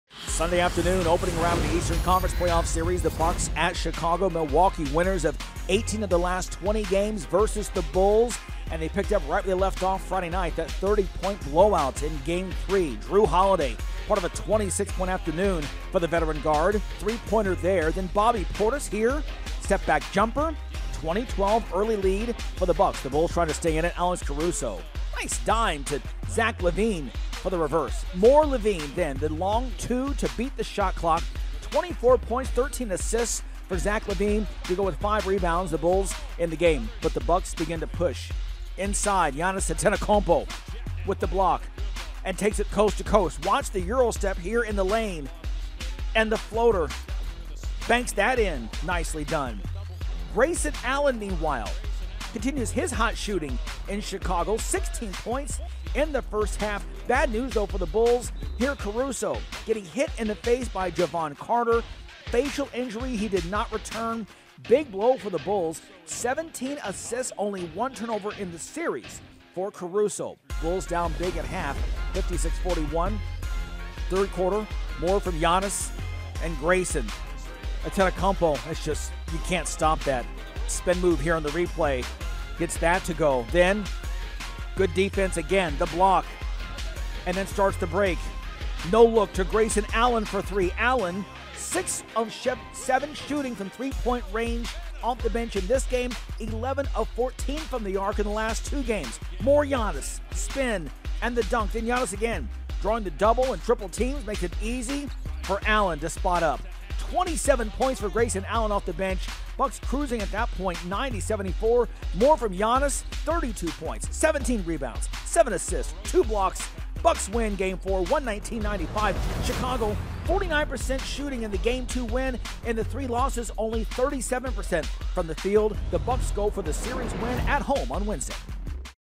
Game Recap: Bucks 119, Bulls 95
Led by Giannis Antetokounmpo's 32 points, 17 rebounds and seven assists, Milwaukee dominates Game 4 to take a commanding lead back to Chicago.